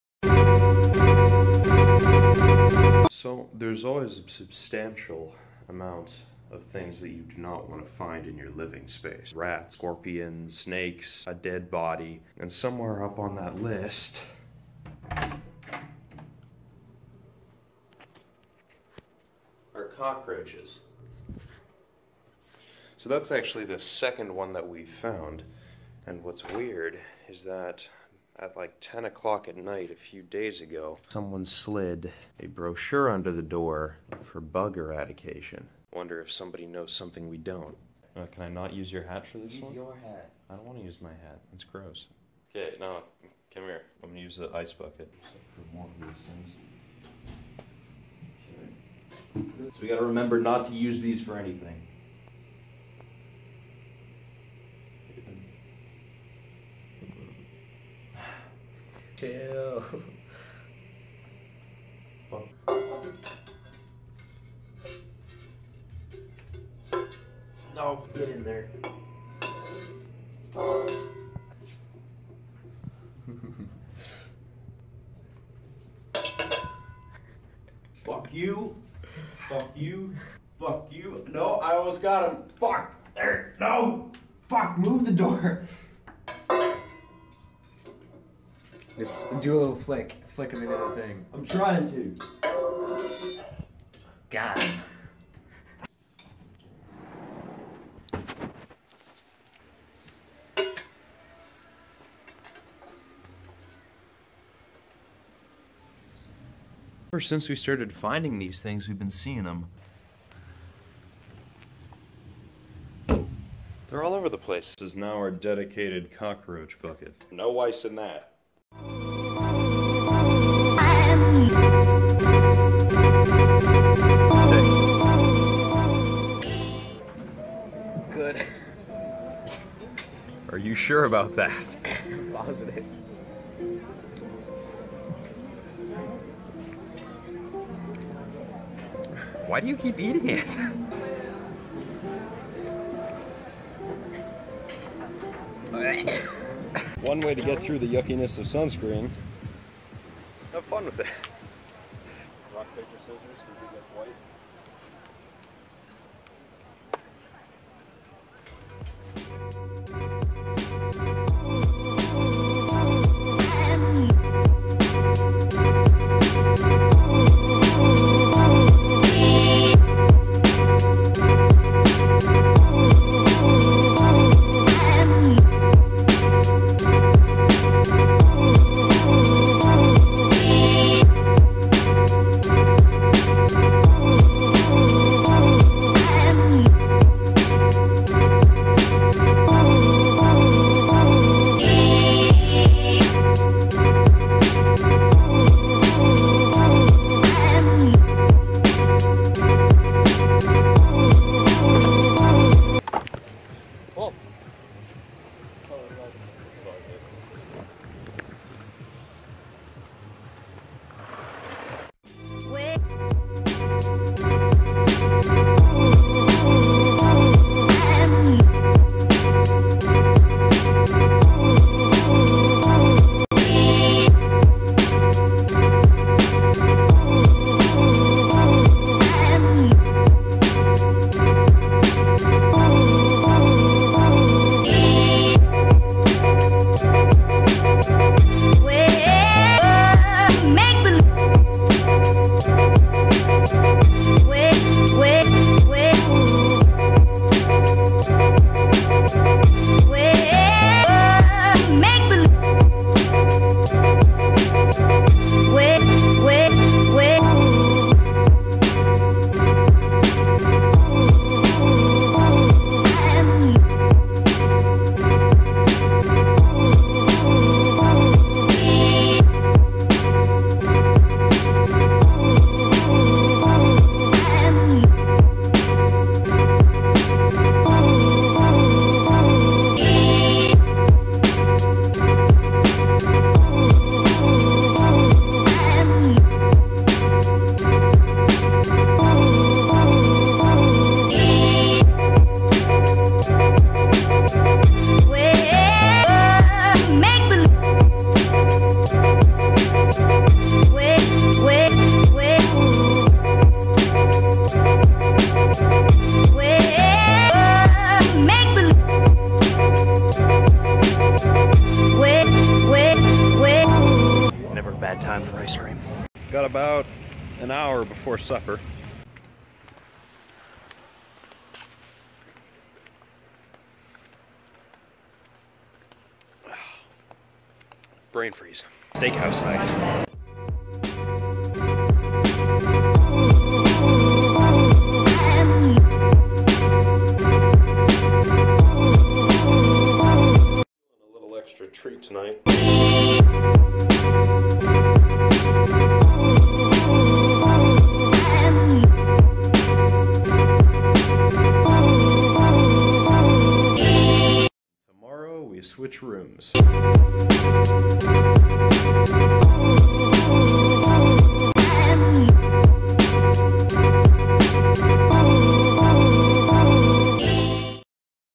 Day 8: Cockroaches & My Brother - Punta Cana Travel Vlog
Day 8. Keep finding cockroaches in the room... Hooray! Otherwise, spent some time with my family on the resort. My brother (Reece) finally beat me in chess for the first time in recorded history.
Check him out on SoundCloud!